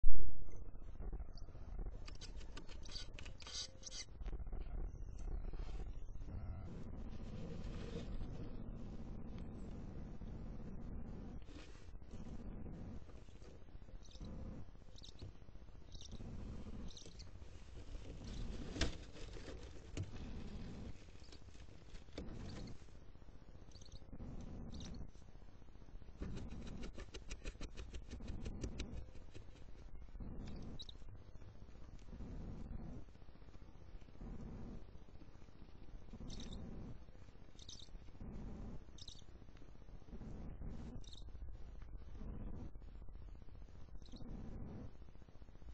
Careful with those pokey toes Mom!
A clip where you can more clearly see hatchling number one.